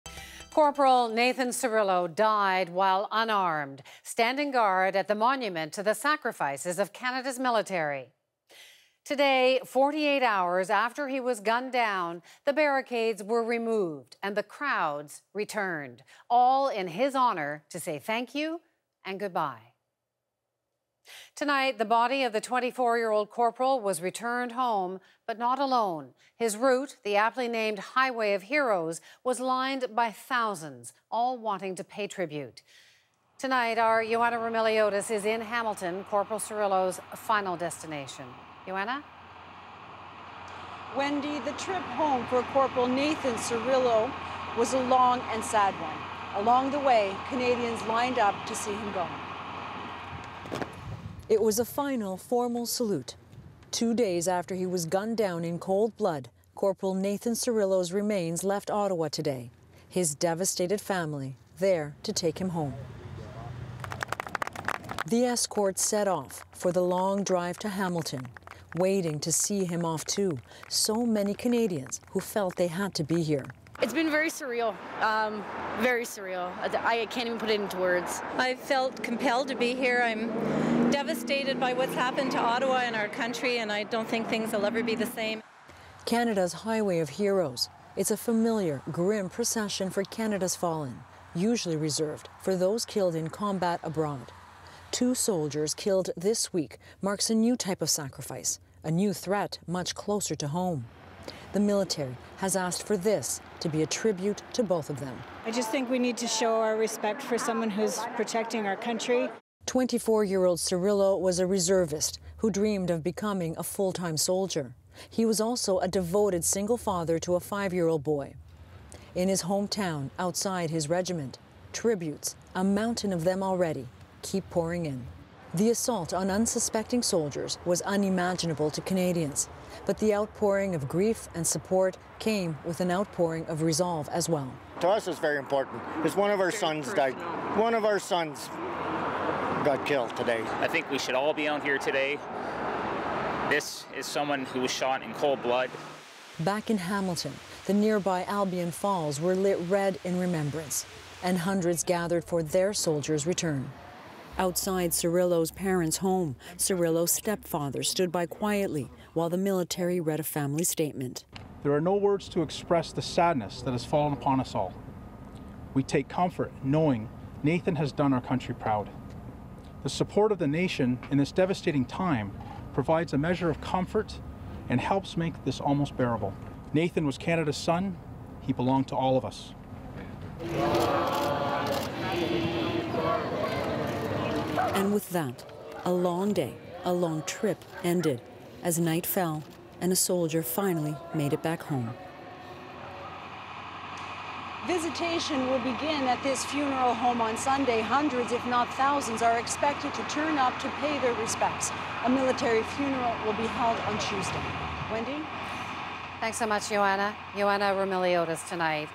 Cpl. Nathan Cirillo's body returned to Hamilton, Ontario
Tonight, the body of Cpl. Nathan Cirillo was returned home via the Highway of Heroes. Canadians lined the route and paid tribute to the slain soldier as the body arrived in Hamilton, Ontario.